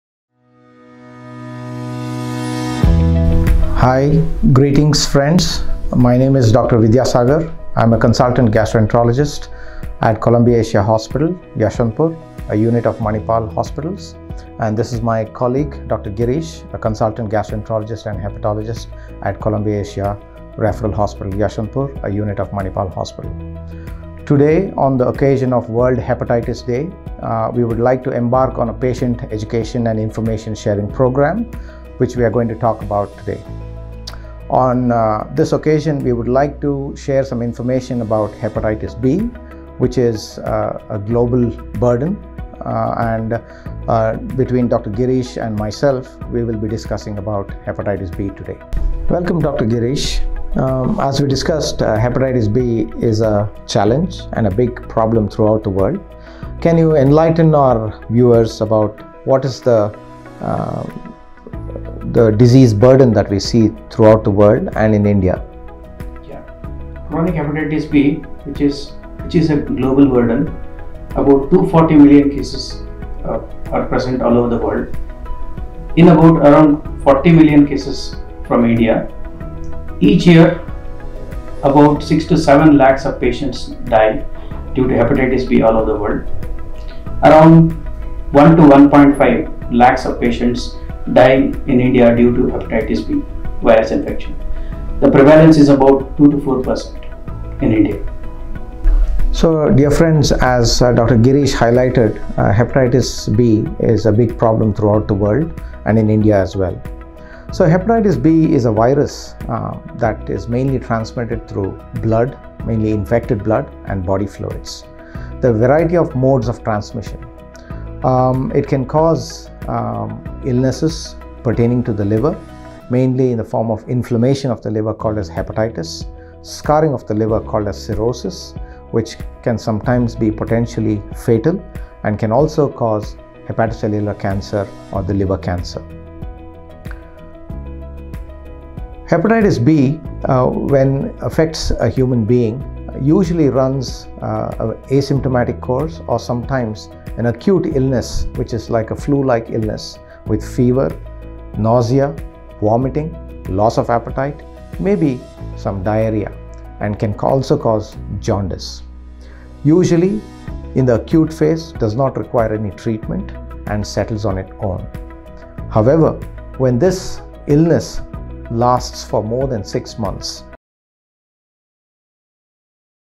World Hepatitis Day 2021 | What is Hepatitis B, | Hep b Causes | Dr. Vidyasagar & Dr. Girish
Dr. Vidyasagar Ramappa, Consultant - Gastroenterology, and Dr. Girish PV, Consultant - Medical Gastroenterology, Columbia Asia Referral Hospital - Yeshwanthpur, A unit of Manipal Hospitals, share their insights on hepatitis B, what is hepatitis b, hep b causes, and what is the fastest way to cure hepatitis b, on the occasion of World Hepatitis Day!